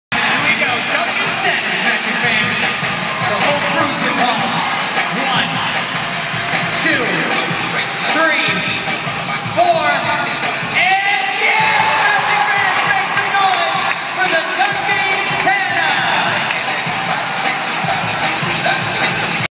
Dunking Santa's at Orlando Magic Game
Halftime dunking Santa's at the Orlando Magic Home Opener.